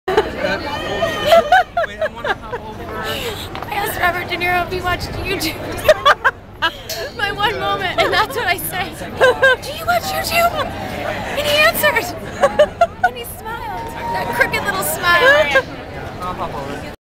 Robert DeNiro talked to me!!
At the Righteous Kill Premiere I got one shining moment with Robert DeNiro..and this is my producer (Kate) and I celebrating!